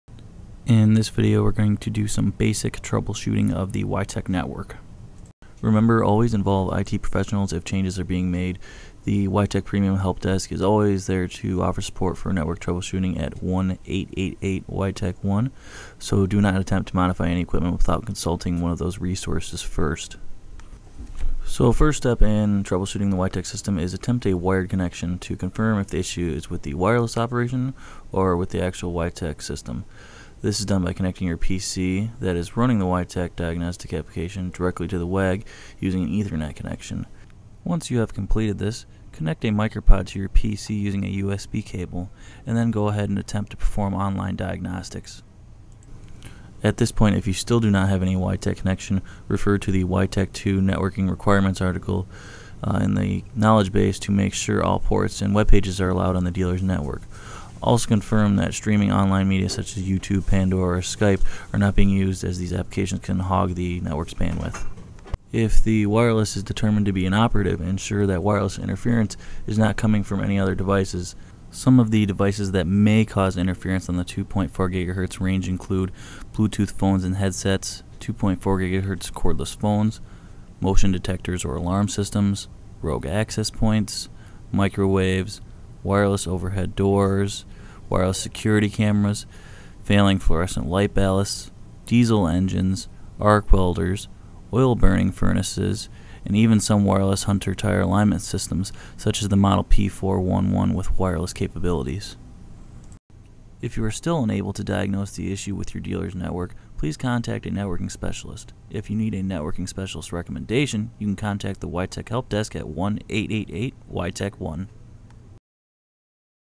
wiTECH Access Gateway Troubleshooting
This video is about network troubleshooting of the wiTECH System. Make sure to always consult professional IT service or the wiTECH Premium Support Help Desk prior to making changes to the network.